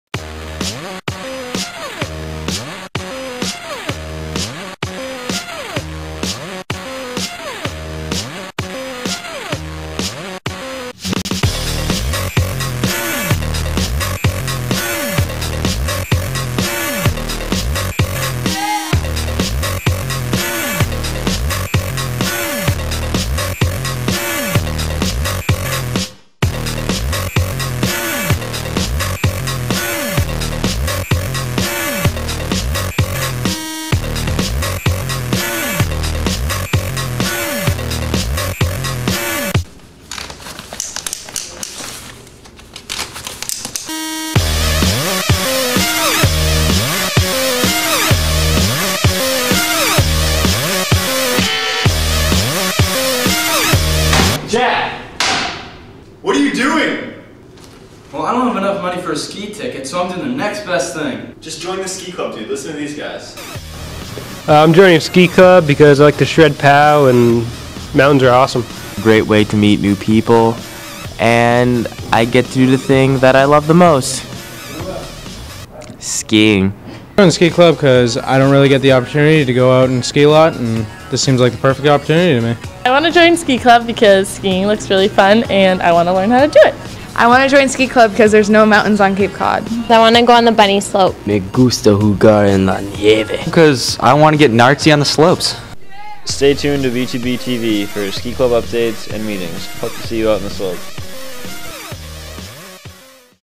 Ski Club Commercial
Ski club commercial for Barnstable High School.
Filmed by Kyle Edited by Jack
Homegrown Productions